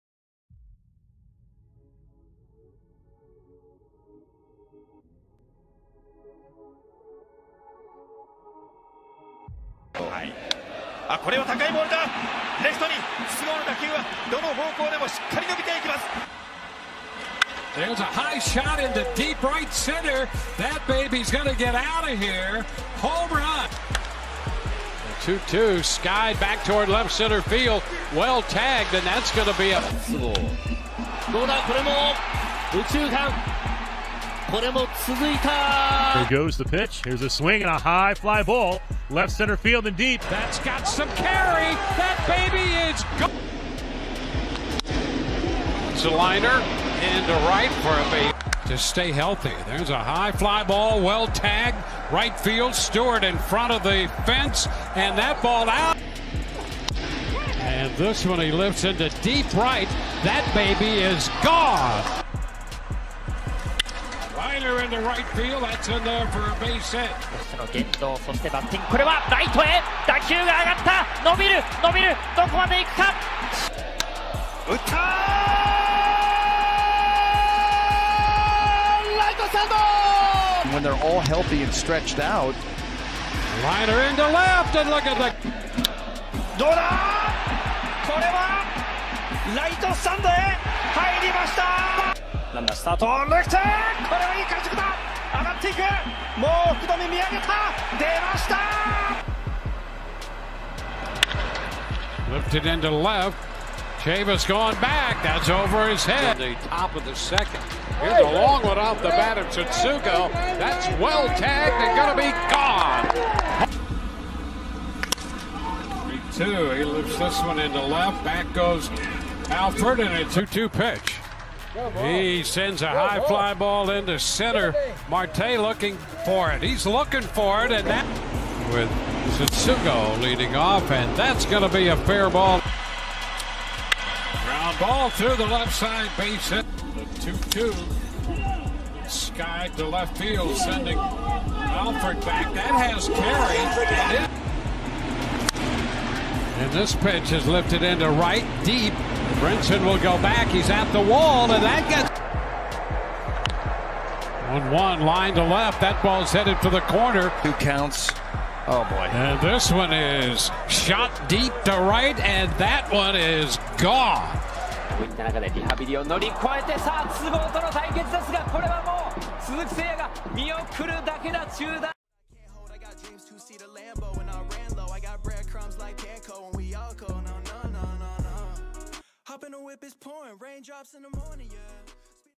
PITTSBURGH PIRATES: Yoshi Tsutsugo ᴴᴰ
BREAKING NEWS || PITTSBURGH PIRATES || YOSHI TSUTSUGO HIGHLIGHTS
89 CAREER GAMES
.181 BATTING AVERAGE
8 HOME RUNS
31 RUNS BATTED IN